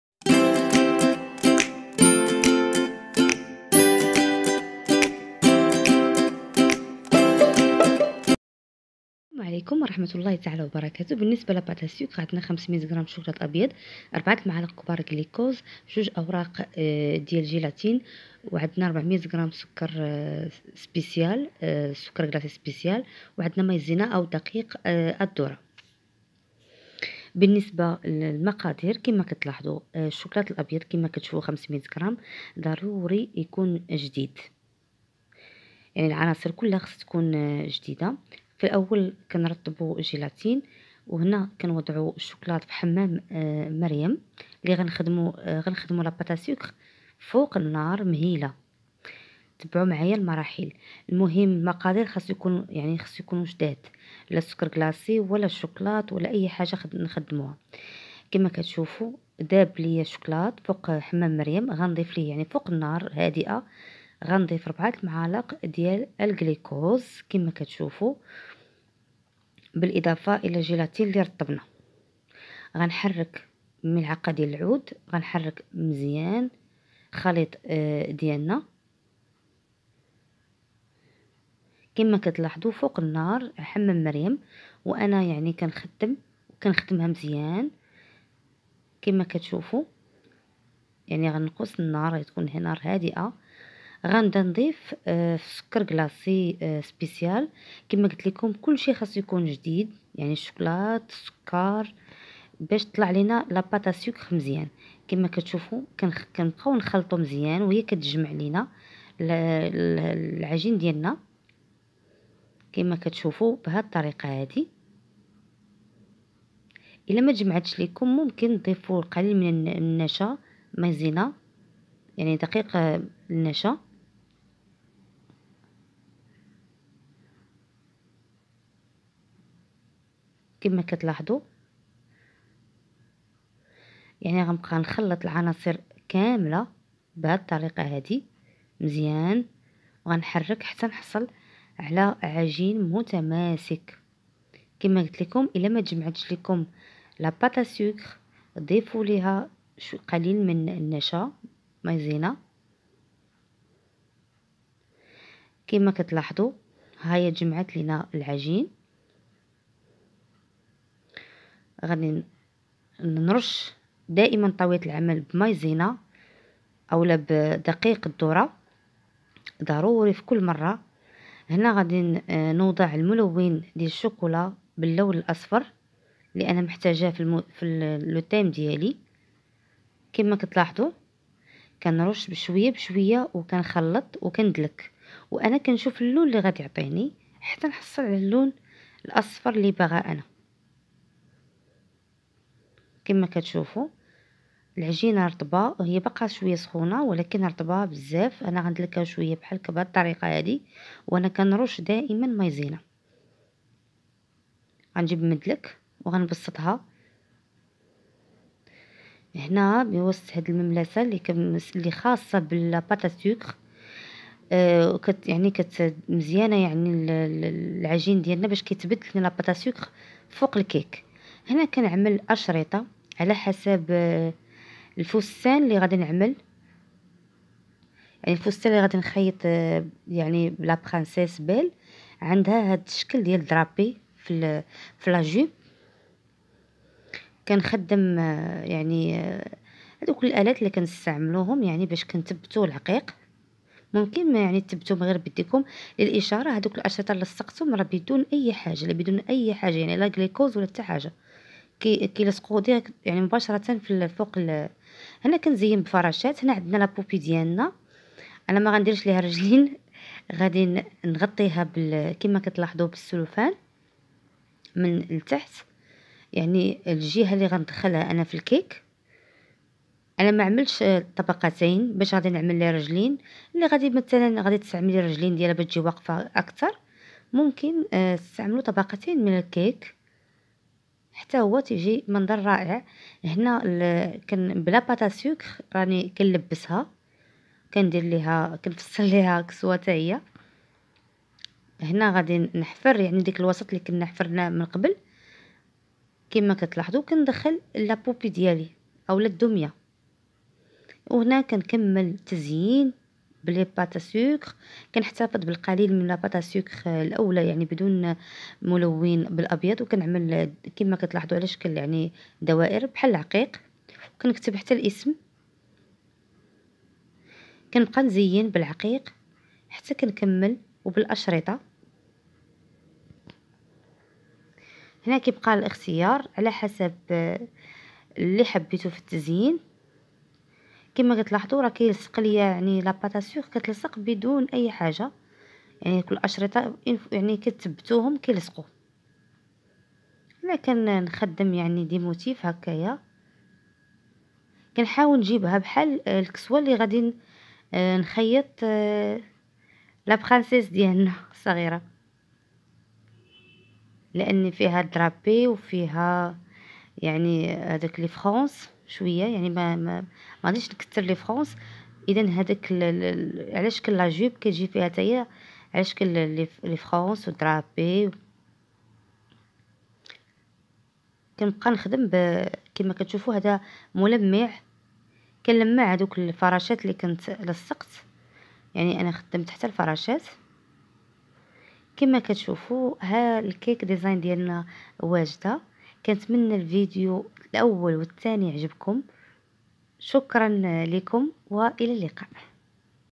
Part2الجزء :cake 🎂design la princesse belle 👩‍🦰كيك الاميرة الجميلة
عجينة السكر:
500gشوكلاط ابيض
400g سكر كلاصي سبيسيال
4معالق كليكوز
2اوراق الجيلاتين
ملون الشوكلاط أصفر
مايزينة او دقيق الدرة
للتزيين : عقيق اصفر  دمية  و بعض الادوات